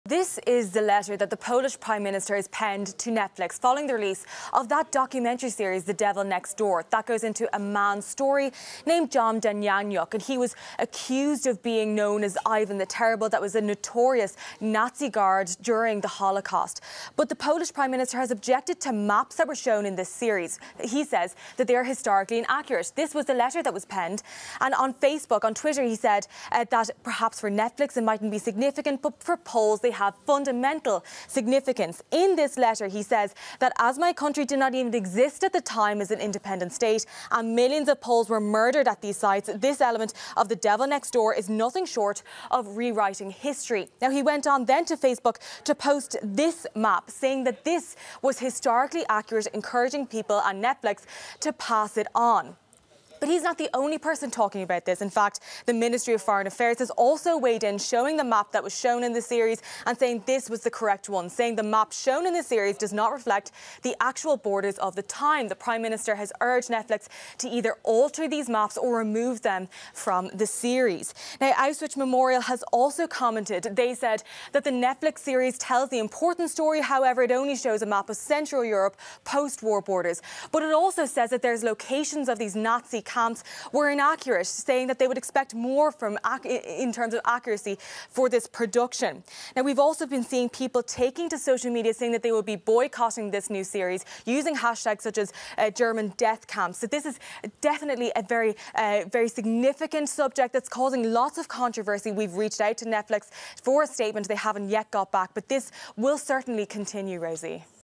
Poland presses Netflix to change documentary about Nazi death camp guard
Poland's government is pressuring the online streaming service to alter a map in its The Devil Next Door documentary series, which shows Nazi death camps inside the border of modern Poland.…